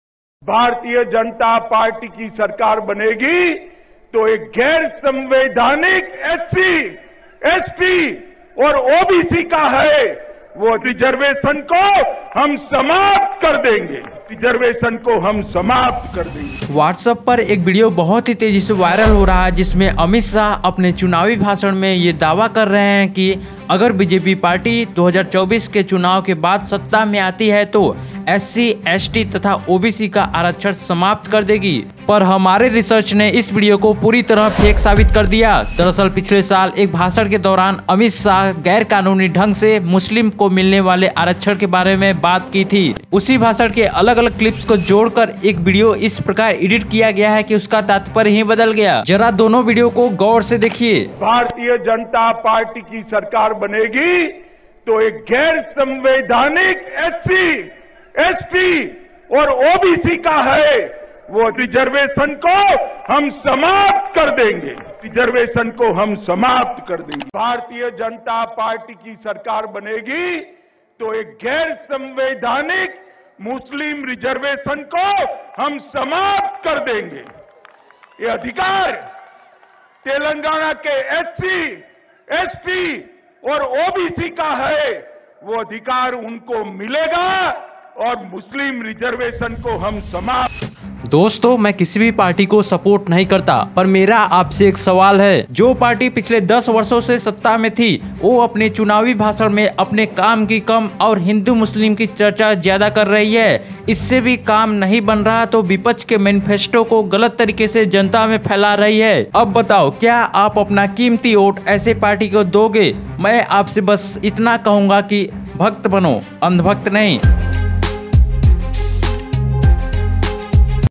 गृह मंत्री अमित शाह का भाषण हो रहा वायरल । Remove reservation of OBC,ST & SC | Dhruv rathee
Amit shah promised if BJP won they will remove reservation of OBC,ST & SC | AMIT SHAH viral speech